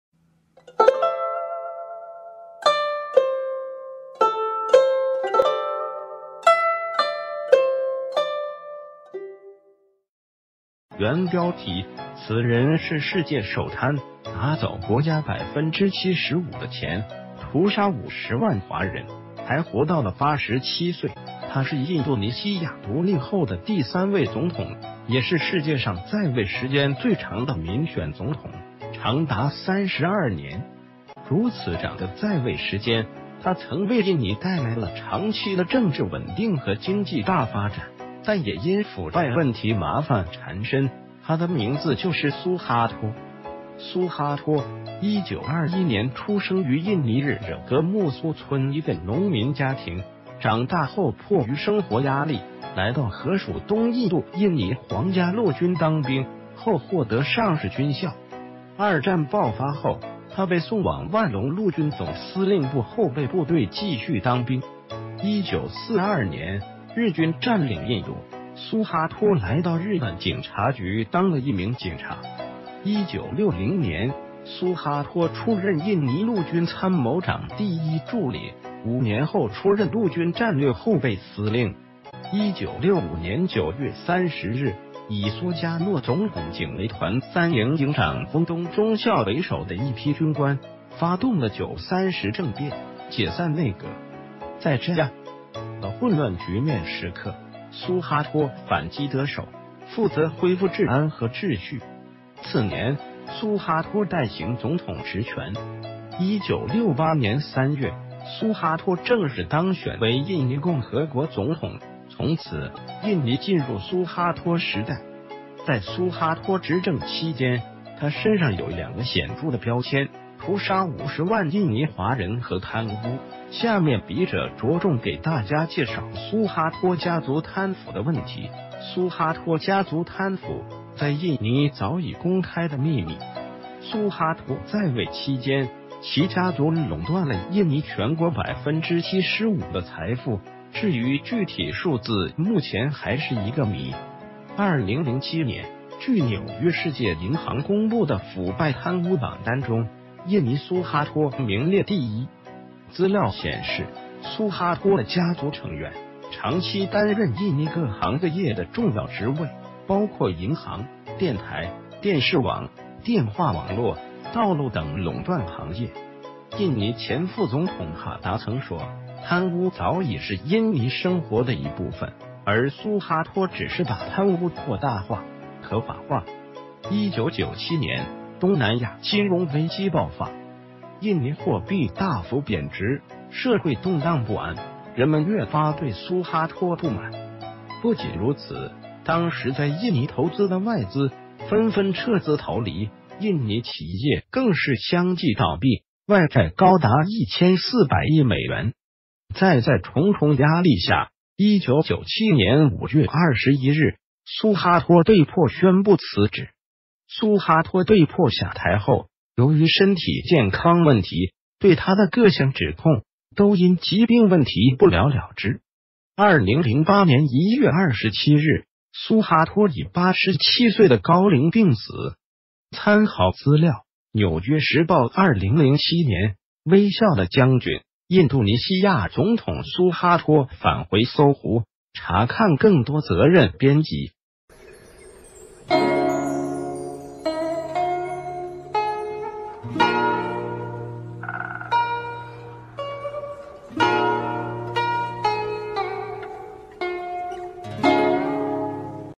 此人是世界首貪：拿走國家75%的錢，屠殺50萬華人，還活到了87歲_蘇哈托
原標題：此人是世界首...